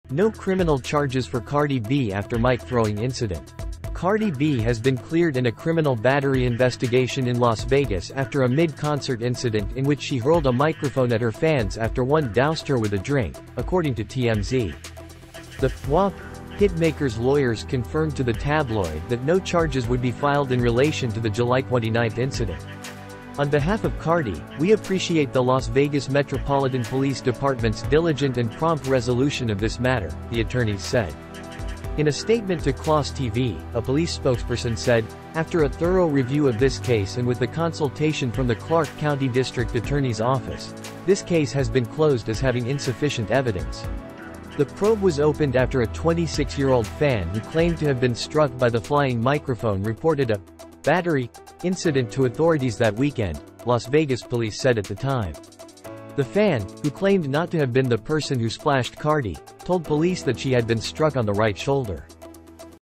No Criminal Charges for Cardi B After Mic Throwing Incident
Cardi B is in the clear after throwing a microphone at a fan ... because the criminal battery investigation has been dropped.

Cardi's lawyers, Drew Findling, David Chesnoff and Richard Schonfeld, tell TMZ, "This afternoon we were notified by the Las Vegas Metropolitan Police Dept., that as a result of their investigation, there will be NO charges against Cardi. On behalf of Cardi, we appreciate the Las Vegas Metropolitan Police Dept's diligent and prompt resolution of this matter."

As you know, Cardi was performing at Drai's Beachclub last Saturday when she asked the crowd to splash water on her to cool her down in the burning Vegas heat ... but when someone threw a cup with an unknown liquid and ice, she hurled the mic at the person. It ricocheted and hit another person, who filed a police report.

Cardi was then listed as a battery suspect but the case has gone nowhere. It was never ever referred to the D.A. The cops concluded Cardi did not commit a criminal offense.

We learned an audio production company that owns the mic decided to auction off the piece, with the money going to charities like the Wounded Warrior Project and Friendship Circle Las Vegas, which helps teens and young adults with special needs. FYI, the bids have hit $100,000.